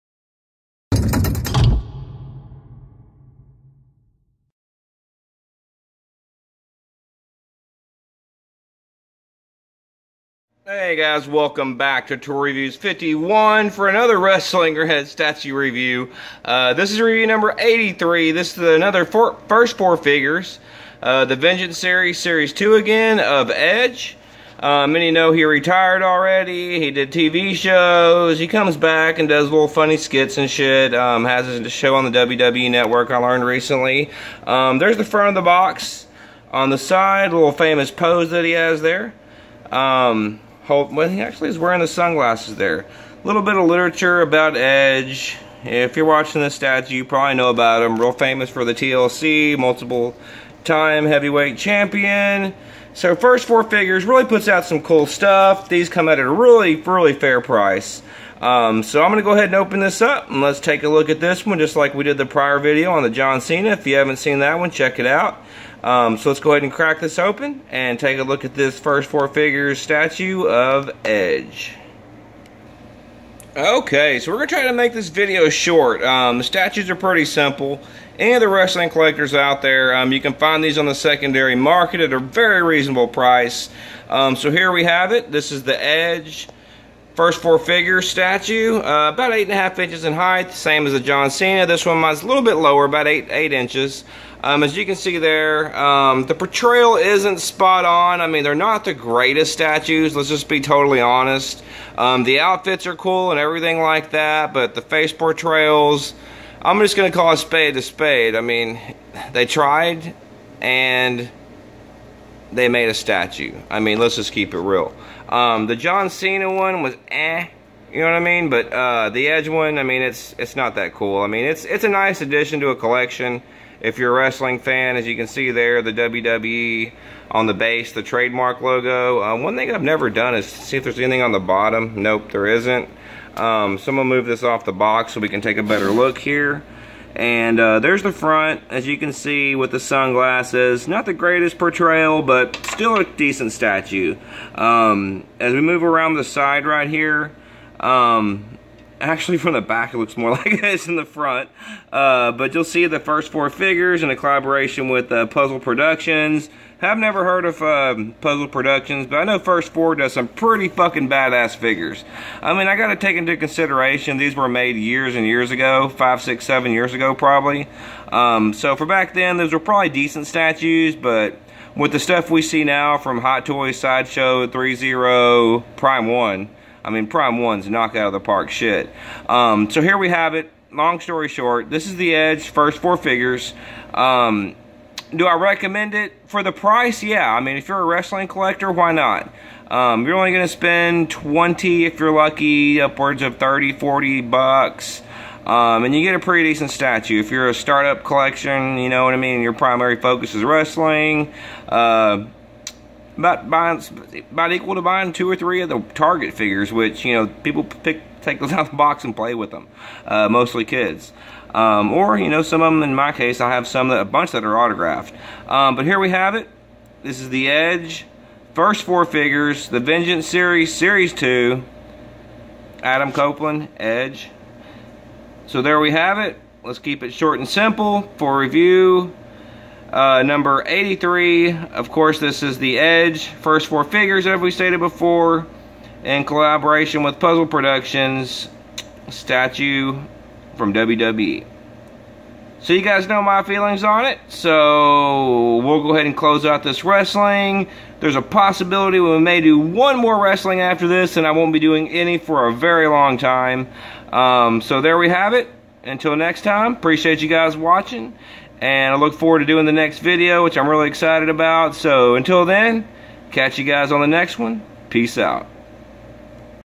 Edge Statue (First 4 Figures) WWE ~ Review #83
Review #83
Edge WWE Statue from First 4 Figures
6.5"-8" Height Statue
Good for a small addition or start up to a wrestling collection.
                                       6/10